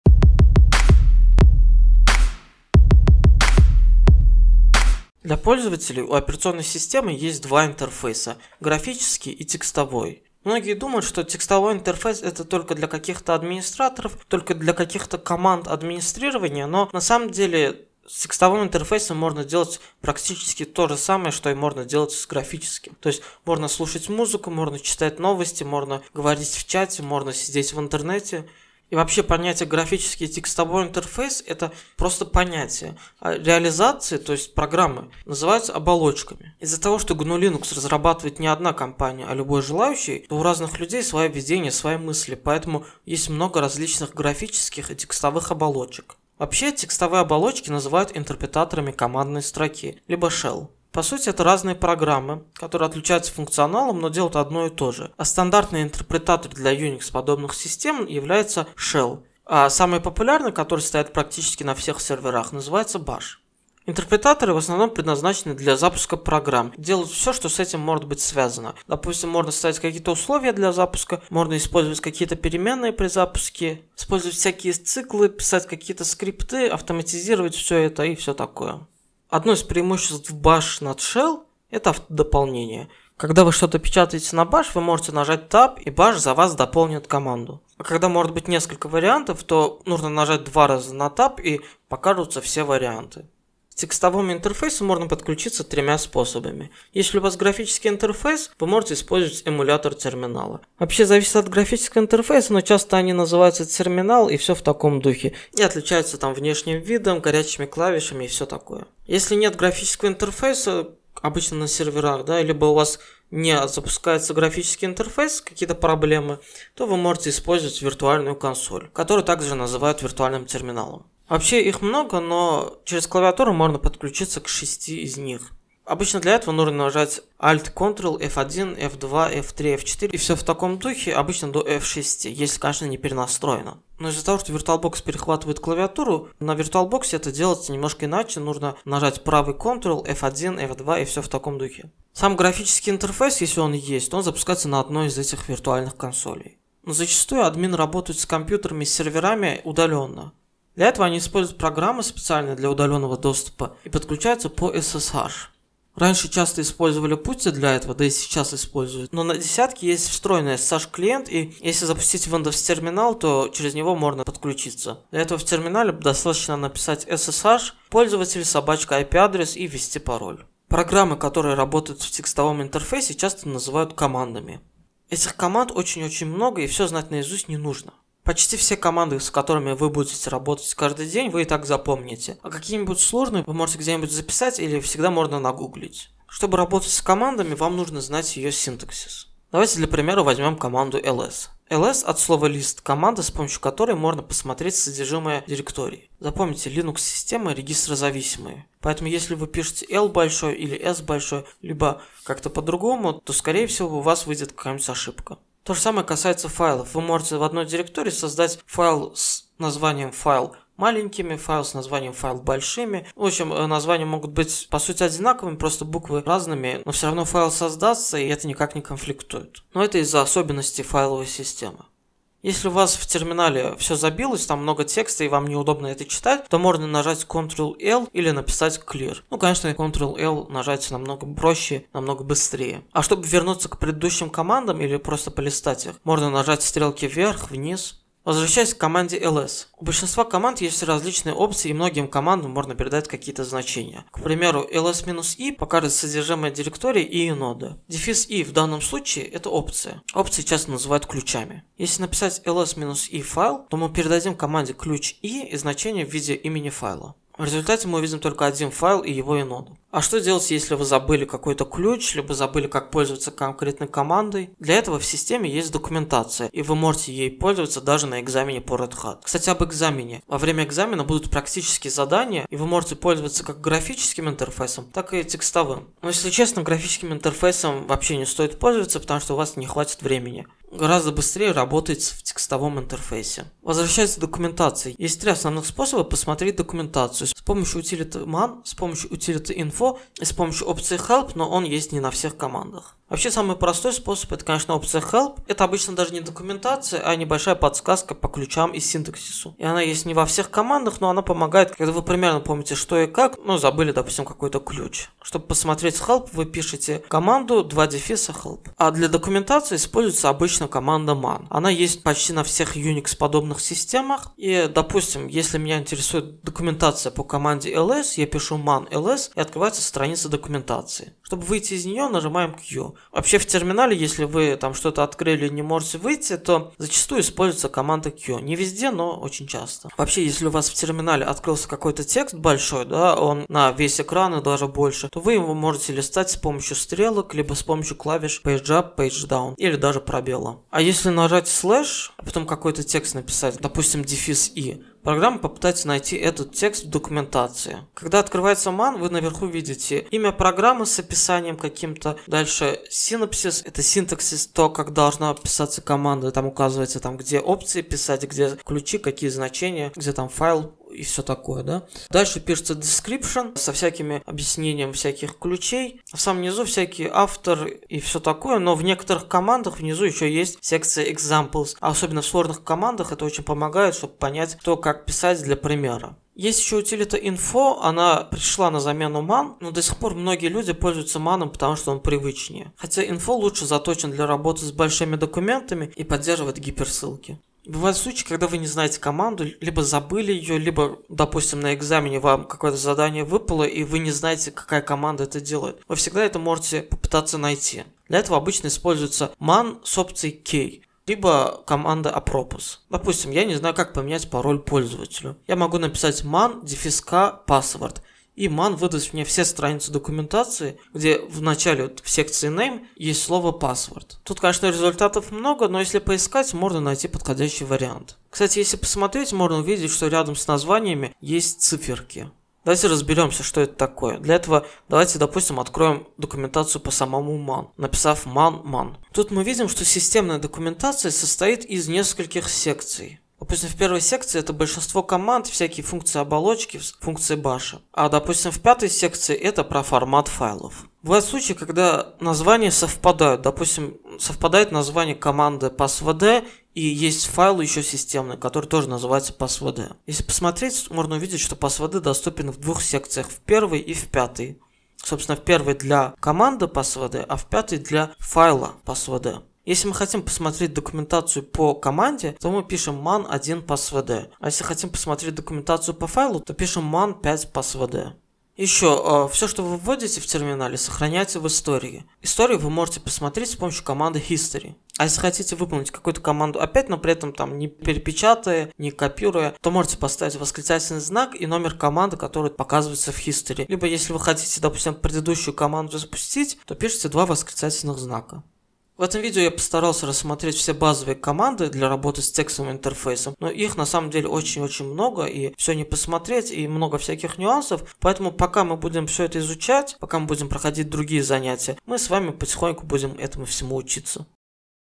05. Текстовый интерфейс пользователя (RHCSA)
То, что я обычно говорю тем, кто хочет научиться работать с линуксом - научитесь читать. GNU/Linux - система, которая всегда что-то пишет пользователю, и если вы научитесь читать и понимать, что хочет сказать система - вы станете профи.